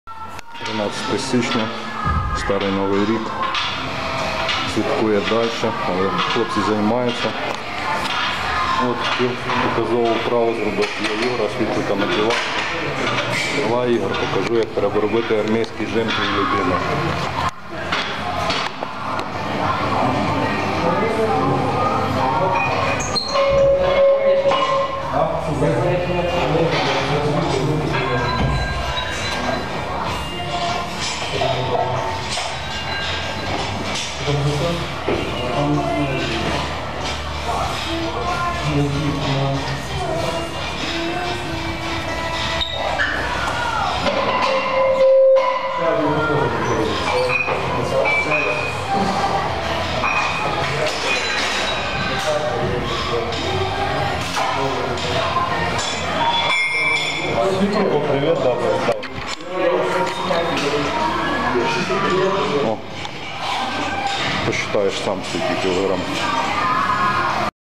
Атлет ТБ 13.01.2015
мотивація для Ігора Світлика - одна із його улюблених вправ - "армєйський жим"